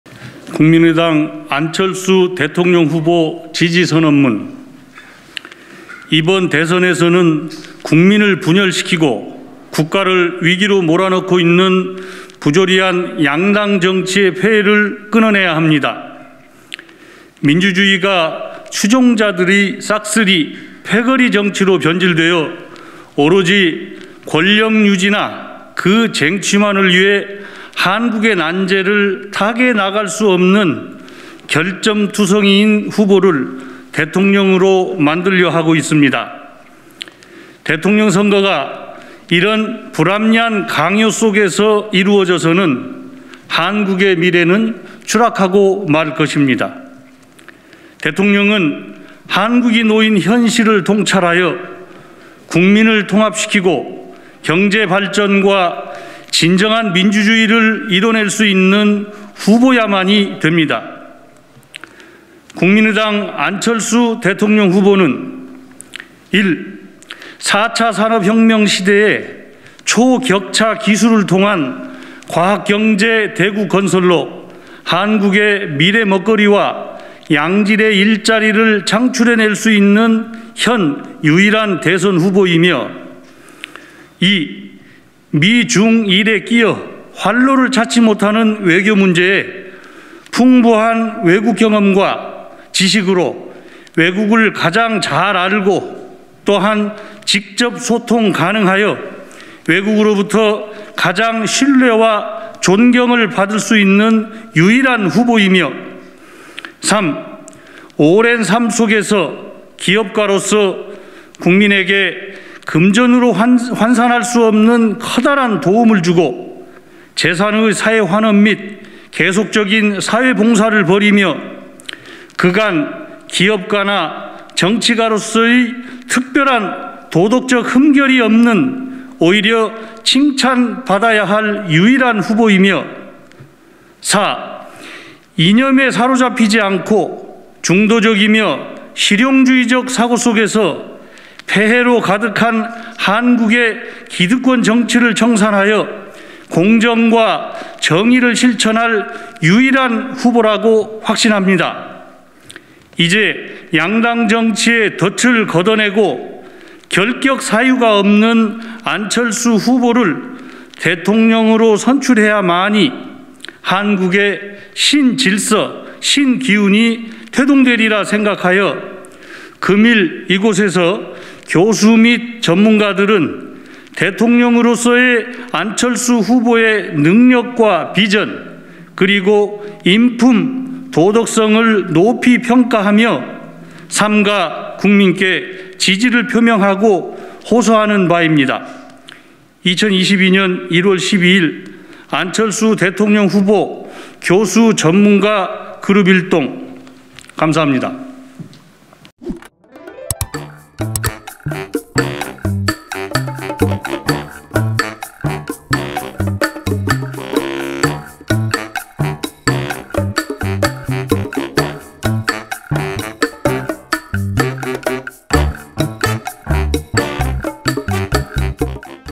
교수·전문가 그룹 "안철수, 공정과 정의를 실천할 유일해"
교수·전문가 그룹이 22년 1월 12일 국회 소통관 기자회견장에서 국민의당 안철수 대선 후보 지지선언을 하고 있다.